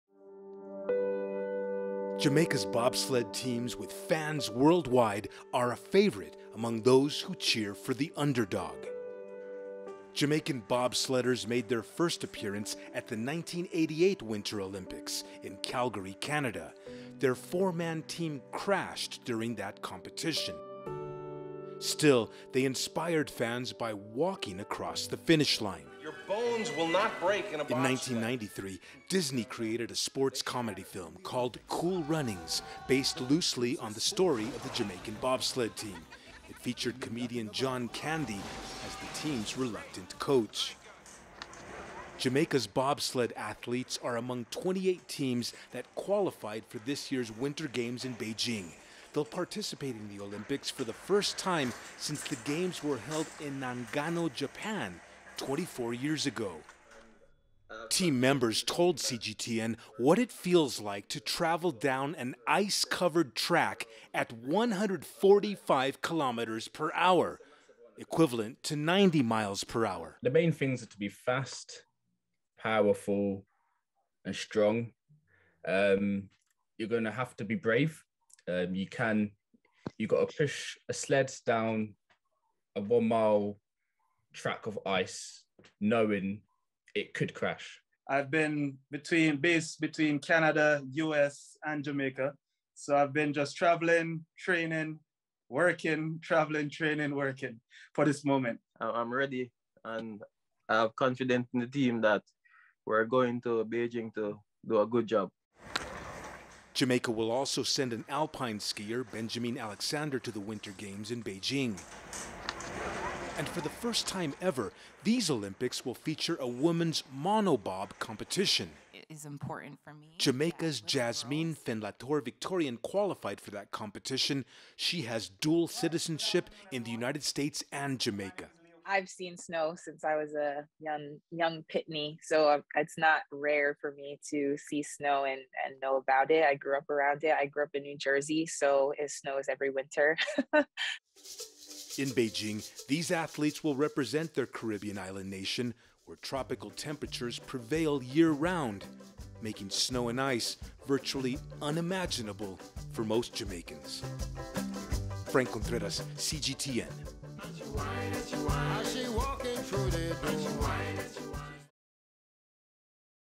Jamaican bobsled team prepares for Beijing Winter Games
During the Winter Olympics in Beijing, sports fans are in for a treat.

The Jamaican 4-man bobsled team will compete for the first time in more than 20 years.

Jamaica will also have a woman racing in the one-woman bobsled competition.

CGTN’s Franc Contreras reports.